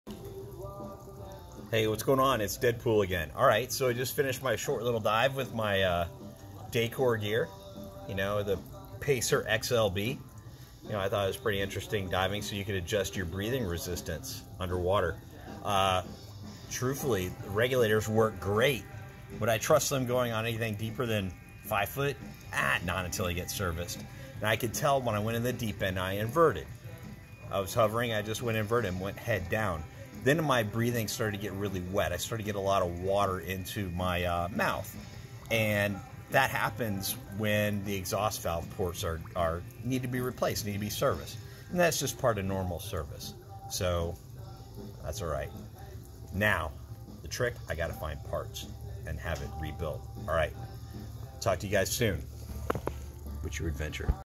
Post Dive with old school gear
This is the post dive wrap up after diving the Dacor Pacer XLB regulator.